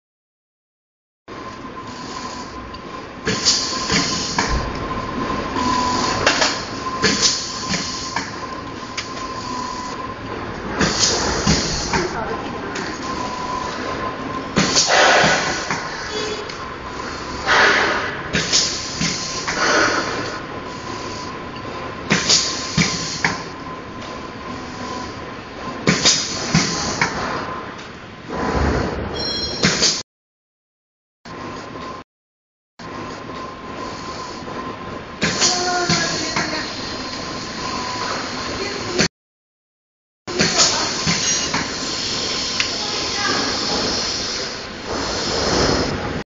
automatic sachet filling and packing machine for honey shampoo tomato sauce
Henan Jingxin Machinery Company